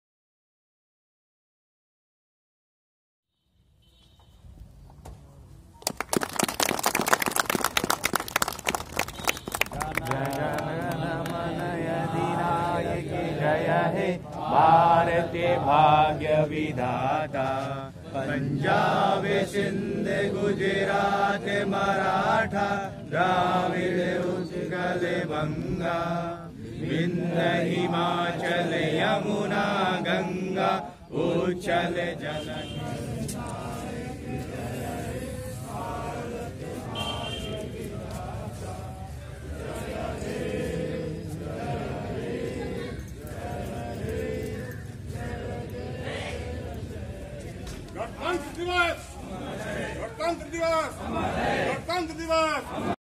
26 January 2021
गोरेया कोठी के विधायक देवेश कांत एवं गोरेया कोठी पंचायत के मुखिया चंदा देवी ने तिरंगा झंडा फहराया